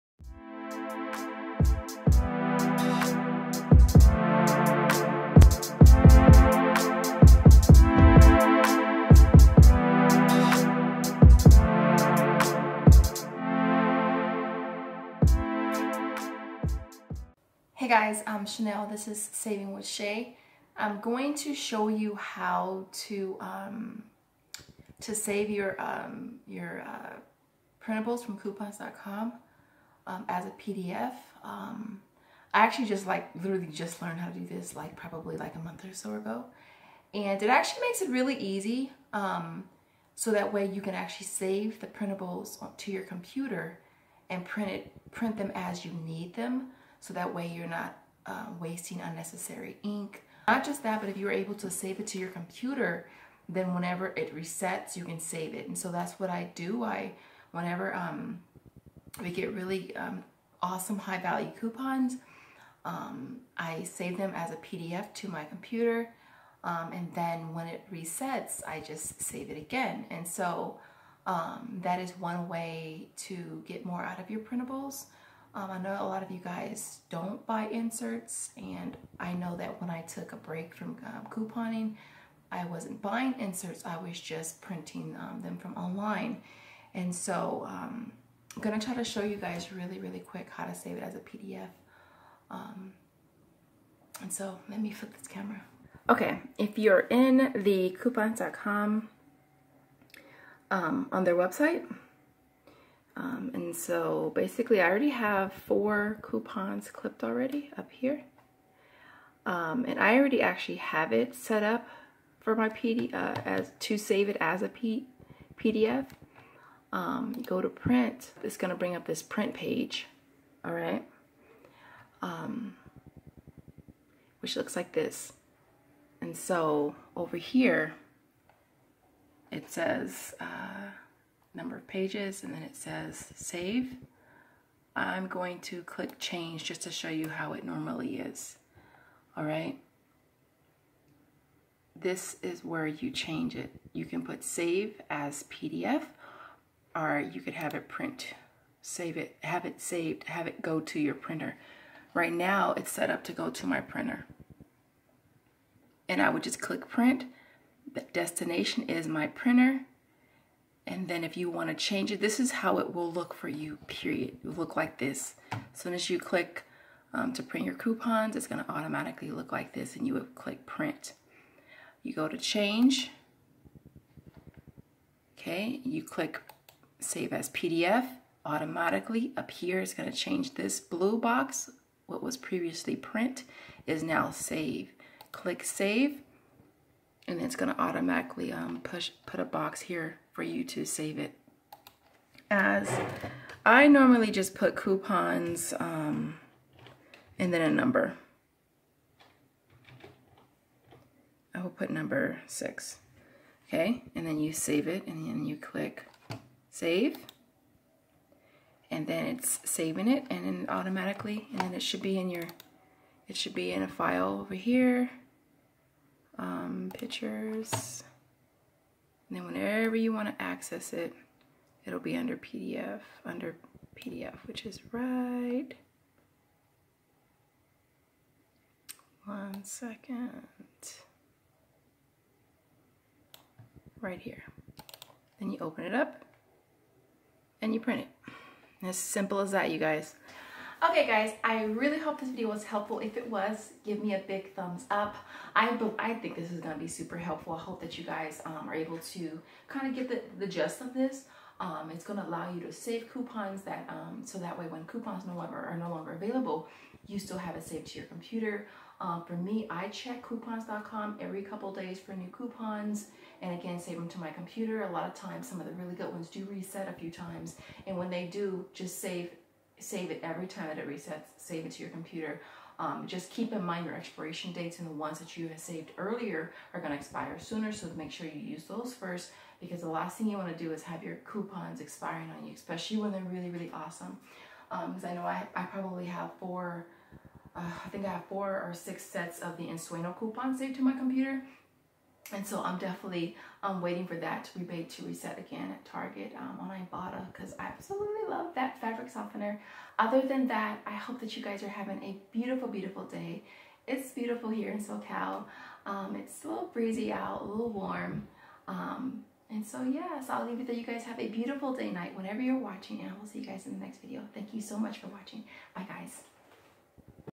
How to Save Coupons as PDF? (On Coupons.com)
Hey guys just wanted share an awesome tip on saving ips to your computer. Really hope you guys find this video helpful. I know this has been so helpful to me.